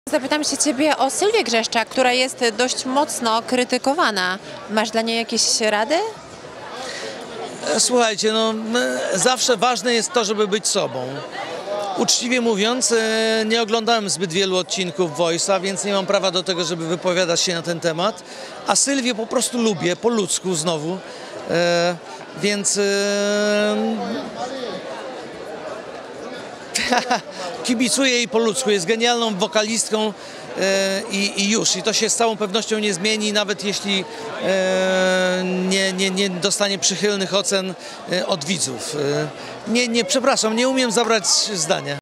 Piasek stanął w obronie Sylwii Grzeszczak. Jakie ma dla niej rady?
Sylwia Grzeszczak w The Voice of Poland zajęła miejsce Andrzeja Piasecznego. Widzom nie spodobała się jednak ta zmiana, a na wokalistkę wylała się fala hejtu. Czy były trener ma dla niej jakieś rady?